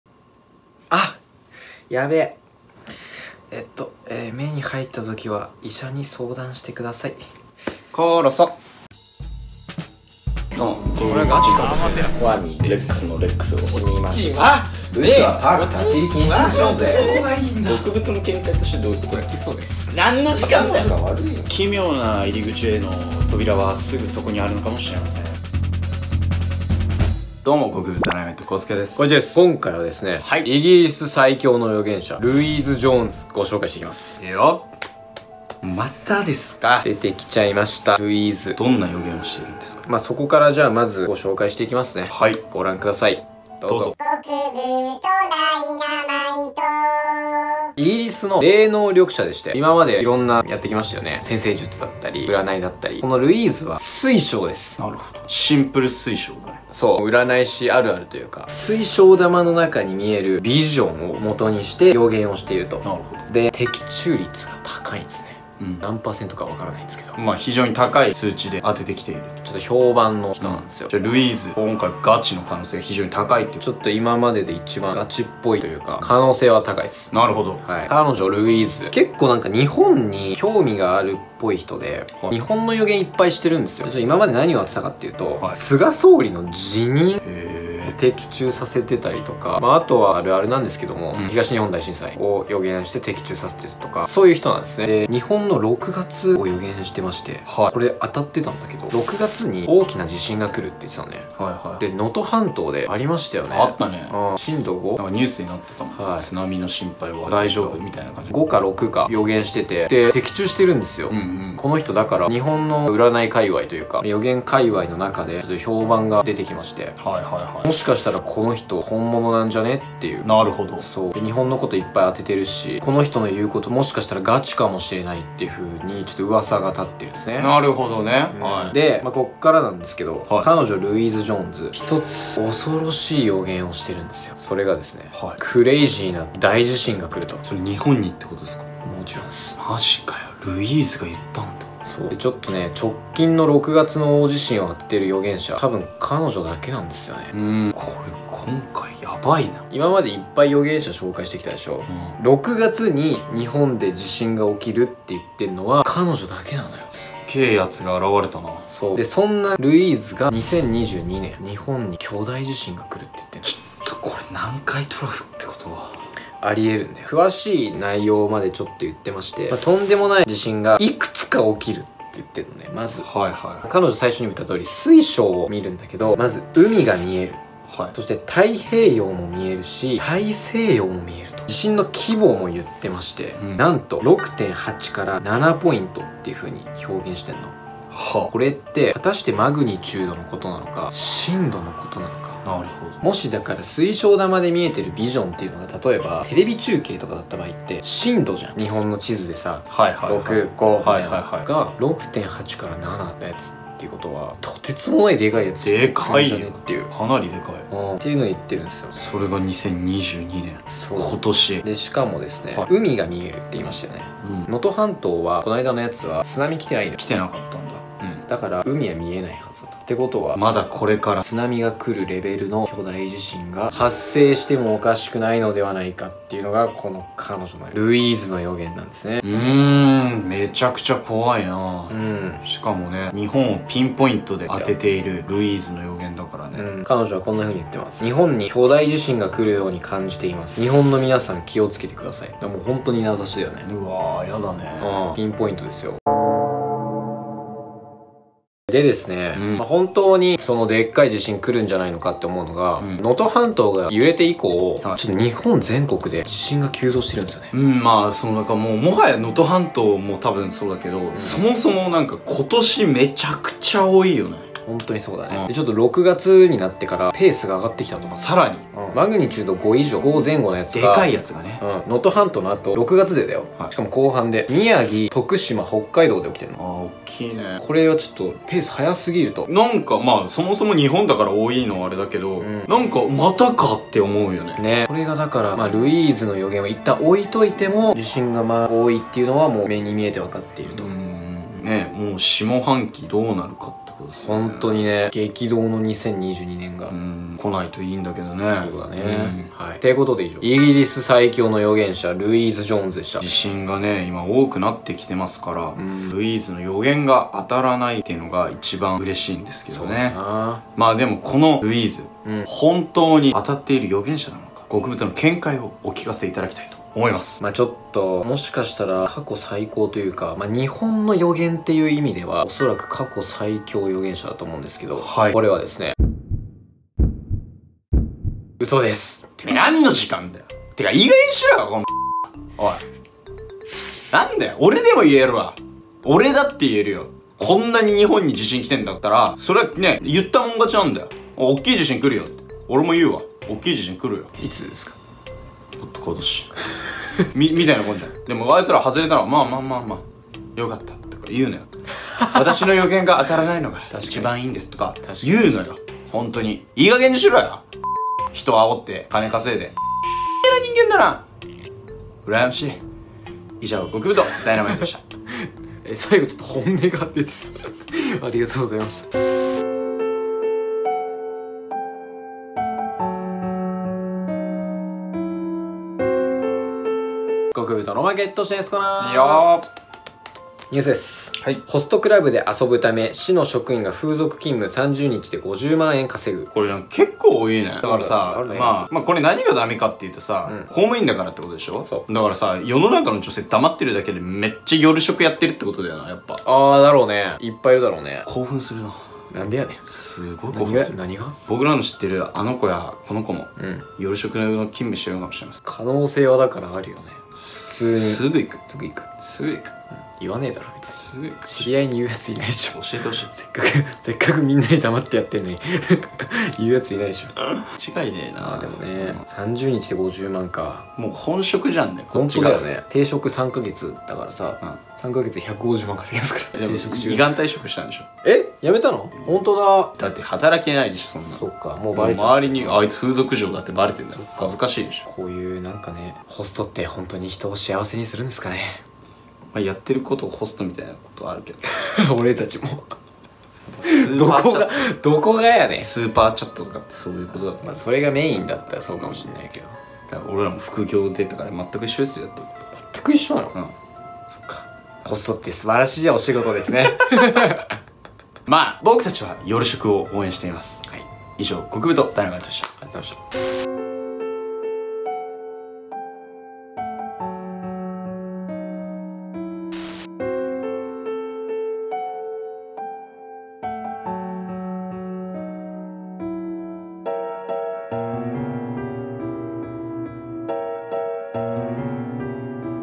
【緊急】2022年中に日本でとてつもない巨大地震が発生します【 予言 ルイーズ・ジョーンズ 南海トラフ 最新 】
イギリス最強の予言者ルイーズジョーンズが見た日本の未来。

ファンアート募集中（動画内で使います）▼